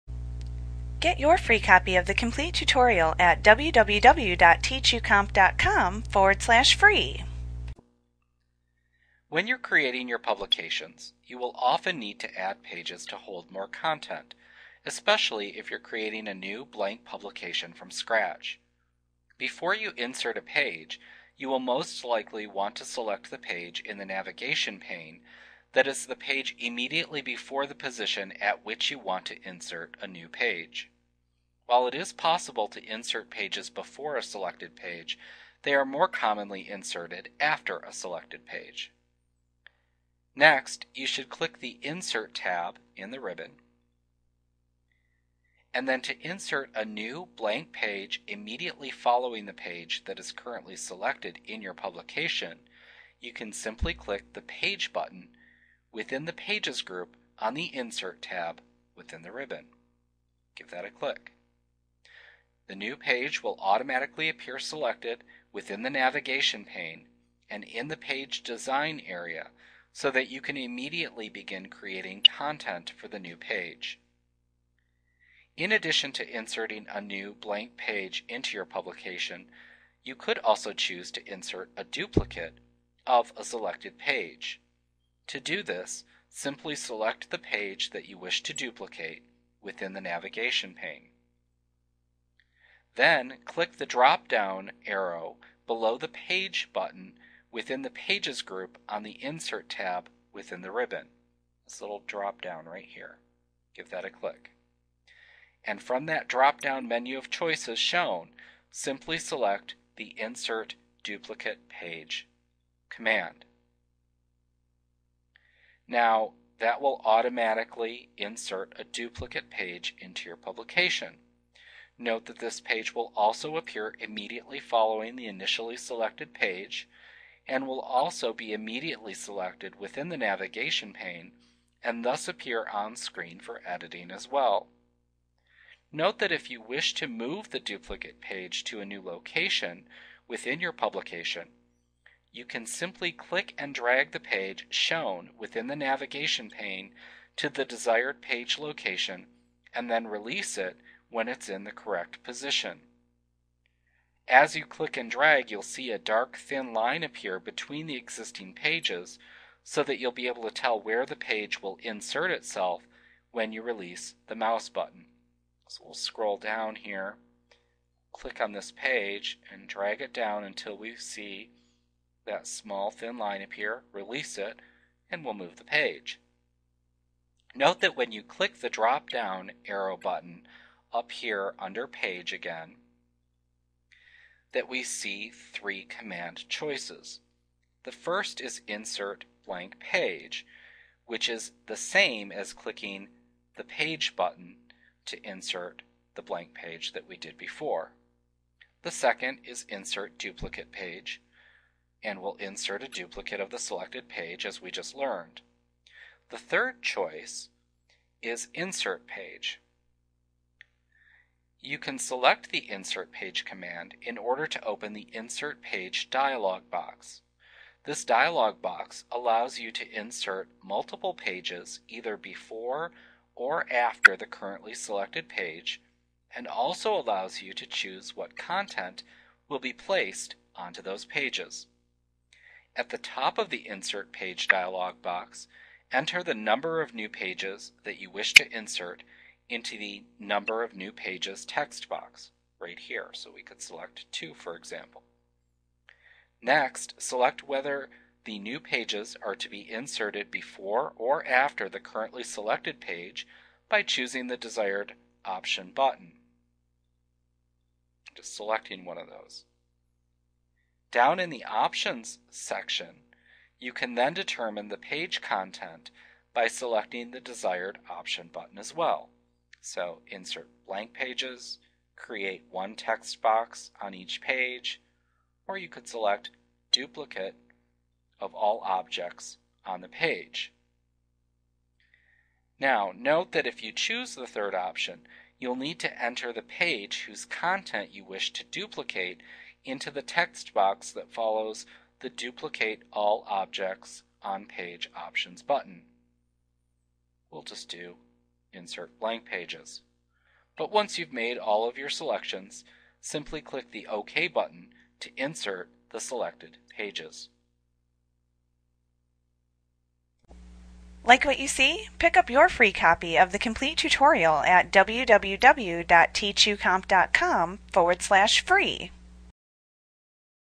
Publisher 2010 Tutorial Inserting New Pages Microsoft Training Lesson 2.6
Learn how to inserting new pages in Microsoft Publisher - the most comprehensive Publisher tutorial available. Visit us today!